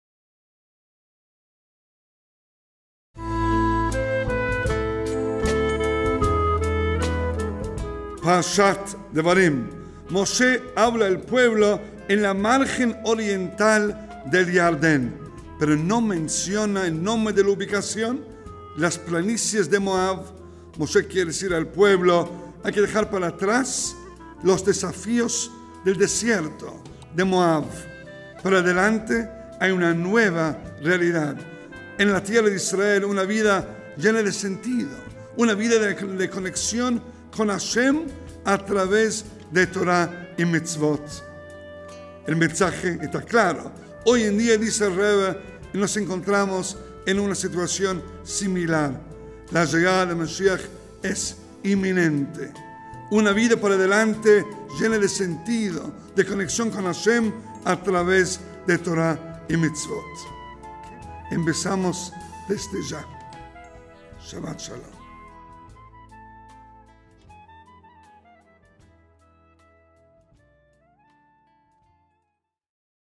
Fechas - Un minuto de Torá (Devarim)
El ya tradicional mensaje semanal del rabino Yossi Turk del Beit Jabad Córdoba